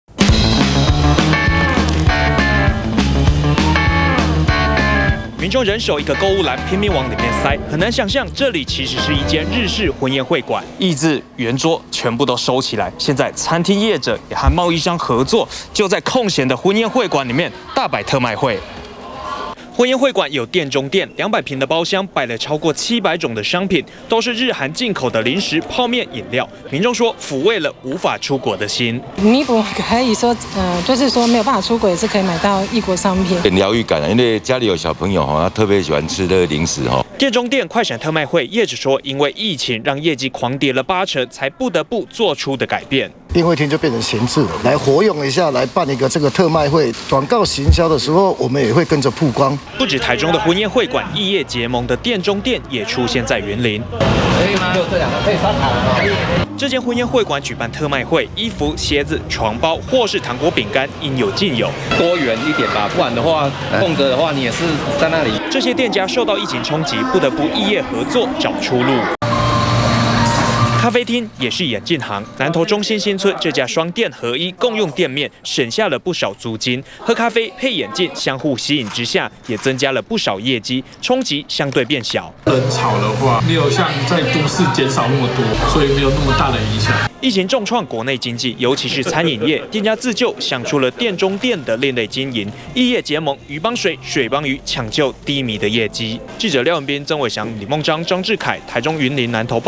20200330中天新聞 婚宴會館「異業結盟」店中店辦特賣救業績
疫情衝擊餐飲業，客人不來，但空間還能租借，台中婚宴會館，包廂開放，進口商辦特賣會，店中店開創商機。